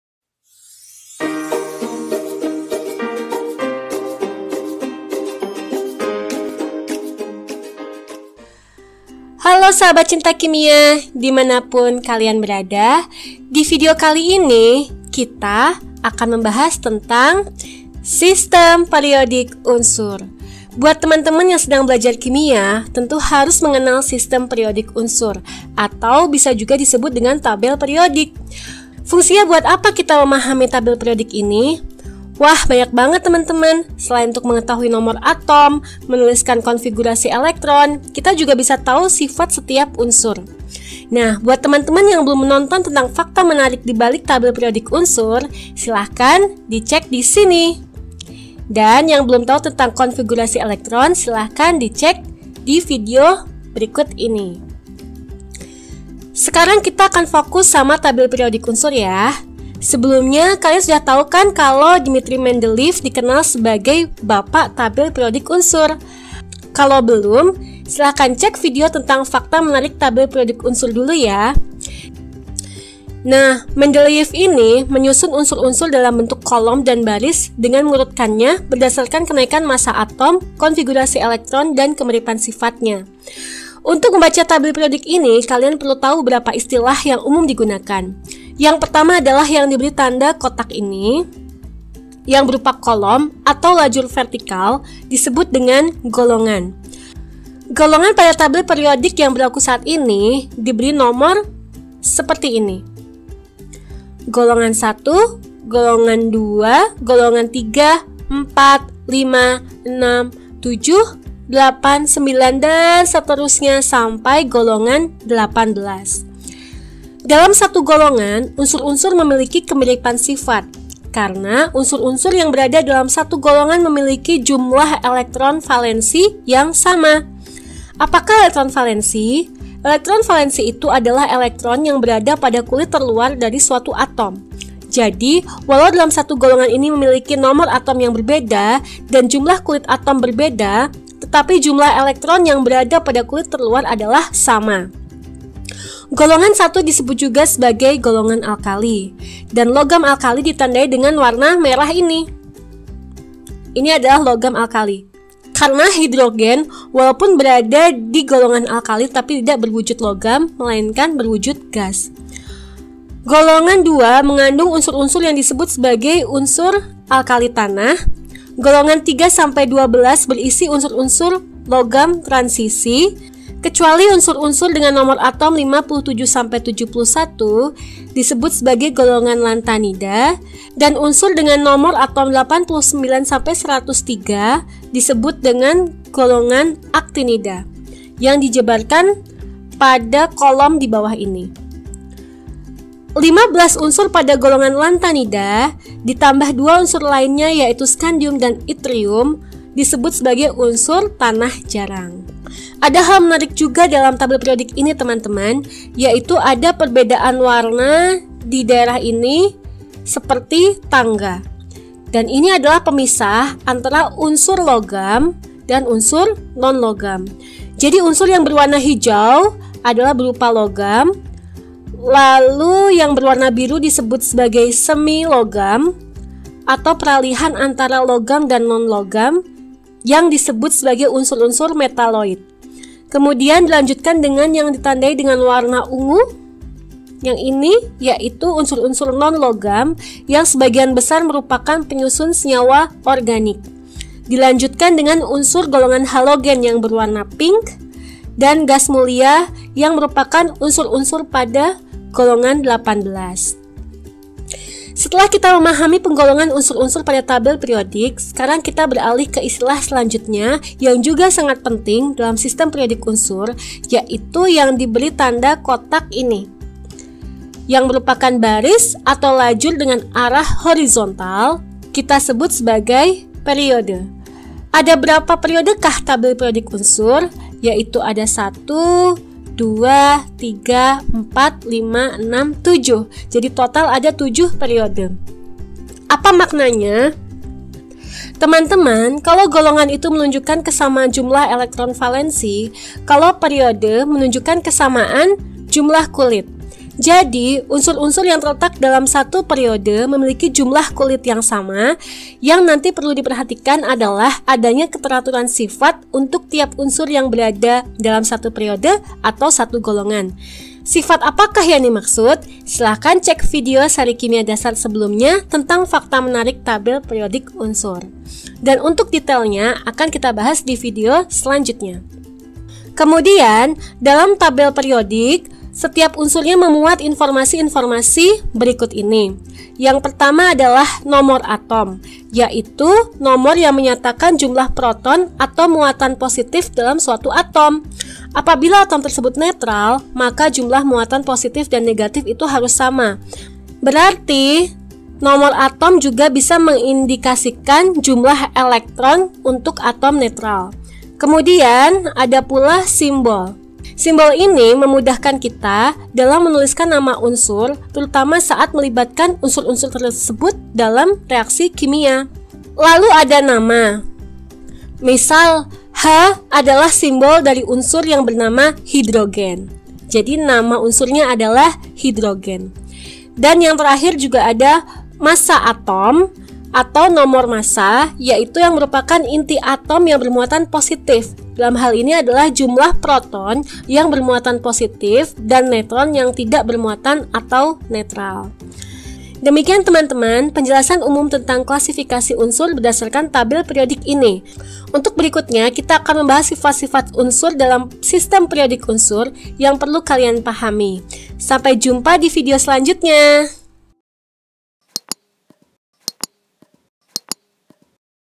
Seri Kimia Dasar - Golongan dan Periode dalam Sistem Periodik Unsur
Hai sahabat Cinta Kimia...

Saat belajar tentang tabel periodik, tentu kata golongan dan periode sudah tidak asing lagi di telinga kalian bukan? Apakah itu golongan? dan apakah periode itu? Mengapa kotak-kotak yang berisi simbol unsur dituliskan dengan warna yang berbeda-beda? Temukan jawabannya di video ini.

Kimia itu indah, kimia itu mudah. Semangat!!! Kamu pasti bisa!!